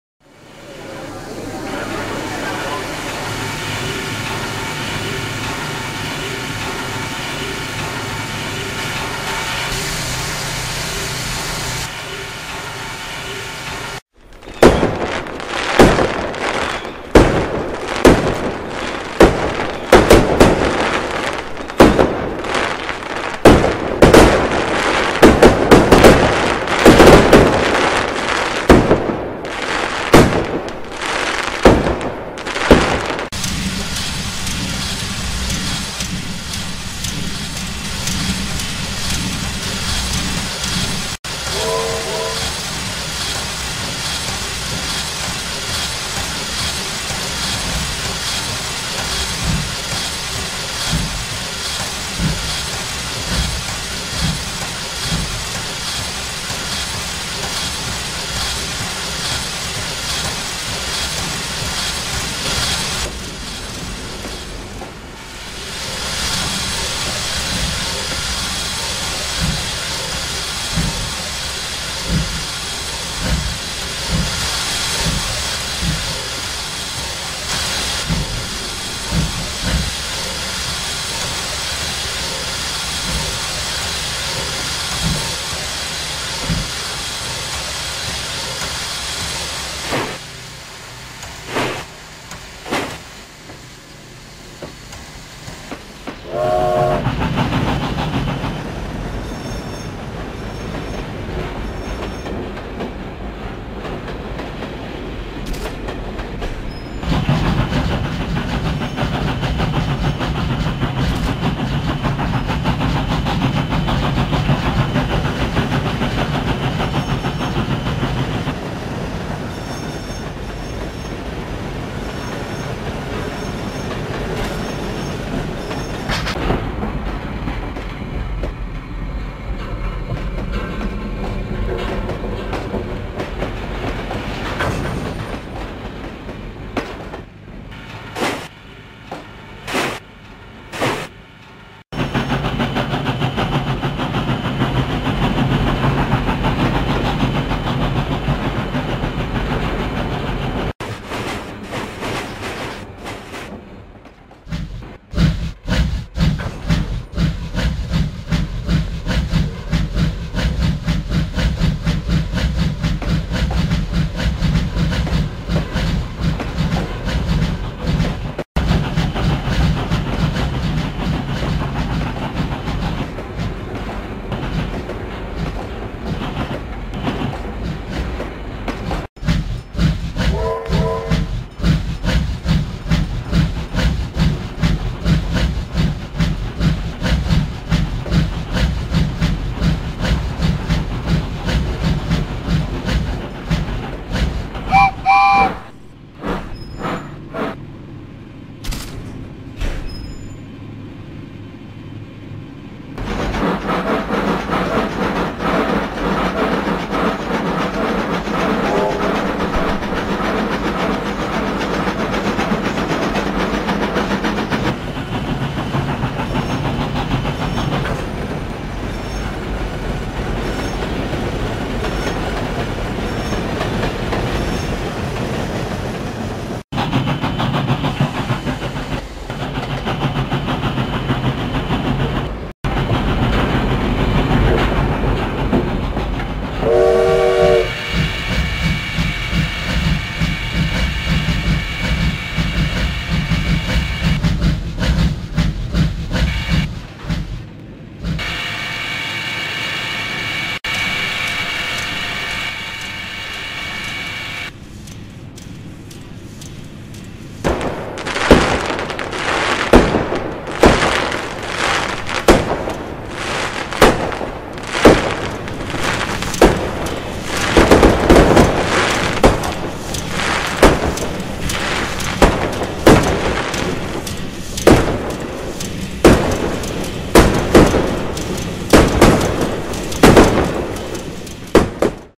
New Zealand Steam Locomotives - Fireworks
Steam Locos:
Ab 776, Ba 552, J 1211, Ja 1240, Ka 945, Ka 942, Wab 794